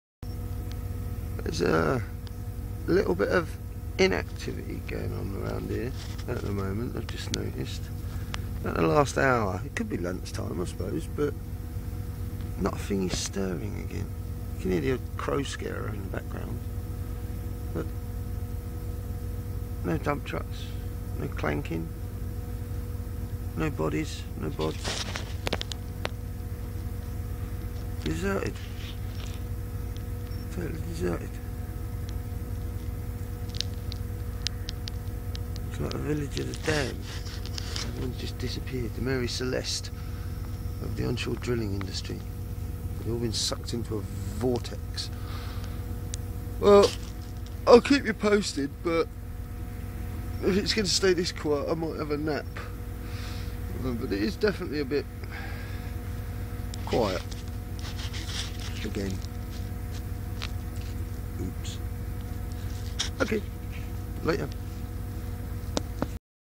Well Intervention gets under way for the second time 11/10/14
11/10/4 The well intervention team set to work making the well safe. Intervention workers actually tell local residents that they are here to make the well safe. The first HONEST statement we have heard from an Agent of Rathlin, or Rathlin themselves.